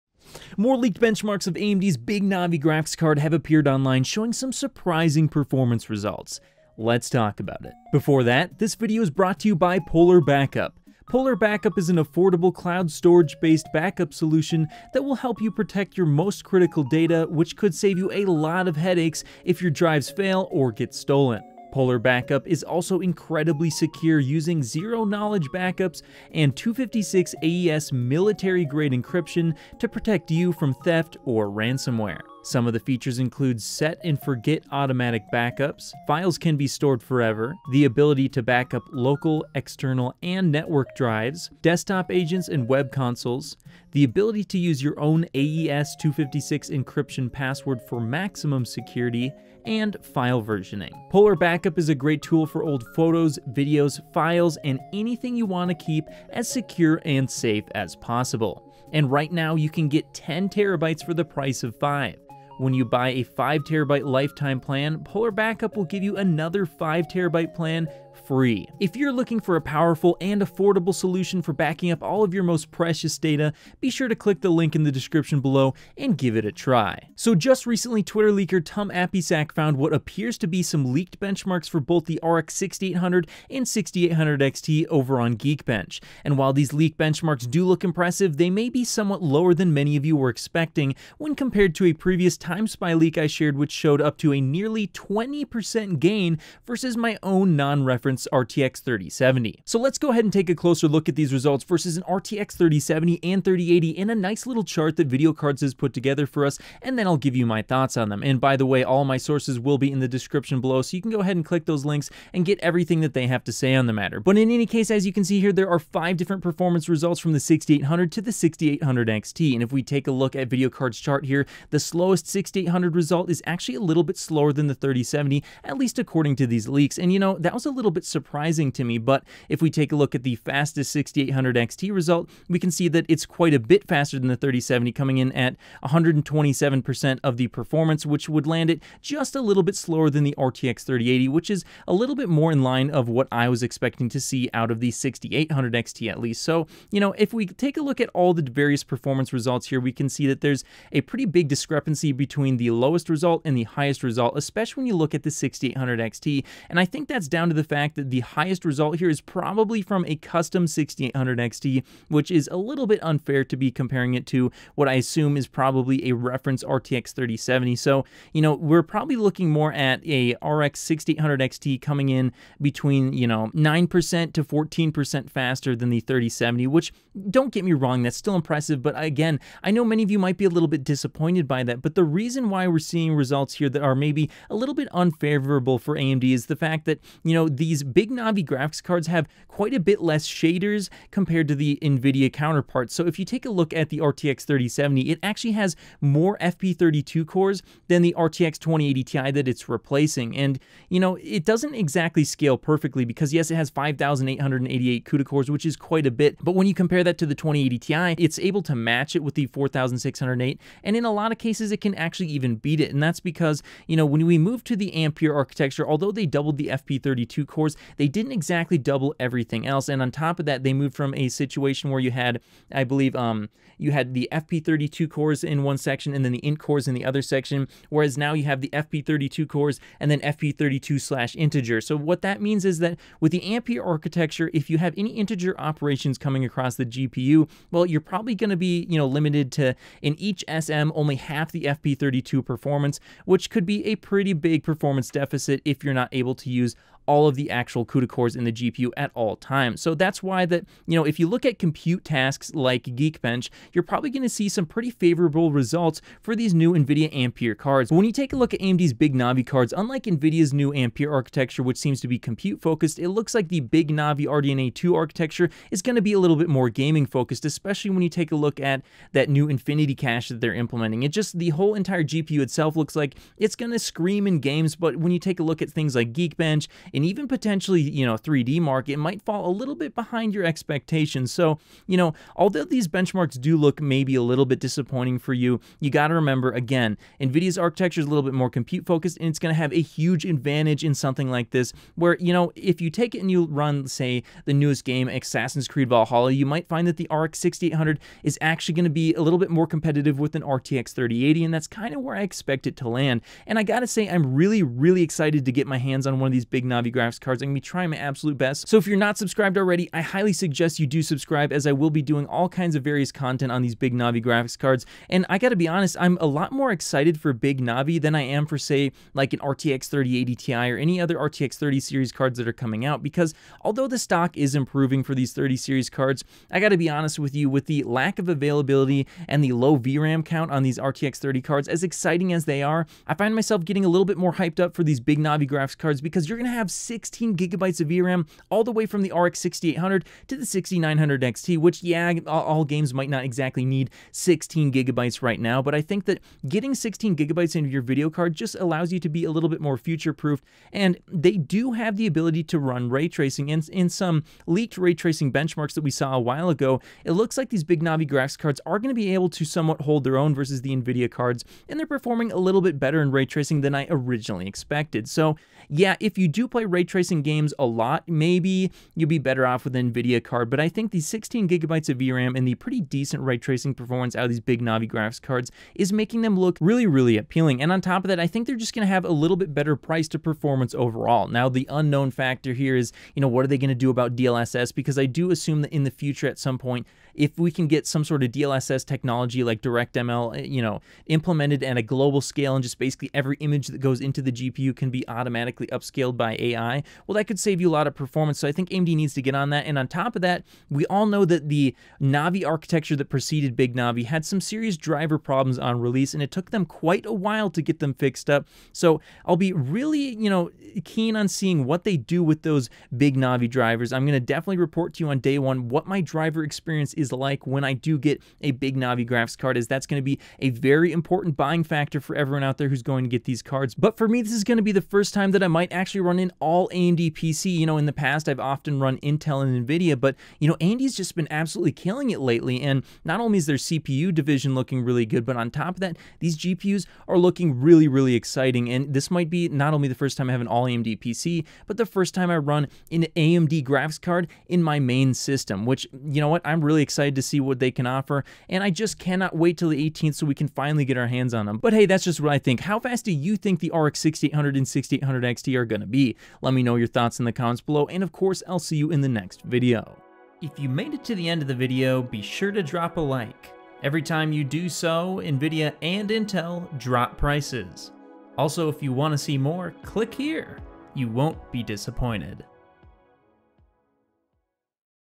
Big Navi RX 6800XT vs RTX 3080 Benchmarks Leak - RX6000 RDNA2
Leaked benchmarks of AMD’s Big Navi RDNA2 Based RX 6800XT and RX 6800 graphics cards have appeared online showing some surprising performance results. According to the leaked benchmarks, The RX 6800 should be roughly on par vs an RTX 3070, and the RX 6800XT should be almost as fast as an RTX 3080.
While these leaked scores do look impressive, they are lower than we were originally expecting for these RX 6000 series Big Navi graphics card. This is likely due to the fact that the RTX 3070 and 3080 are based on the new compute focused Ampere architecture, and Big Navi is based on the gaming focused RDNA2 architecture.

Try out Polarbackup and get 10TB for the price of 5!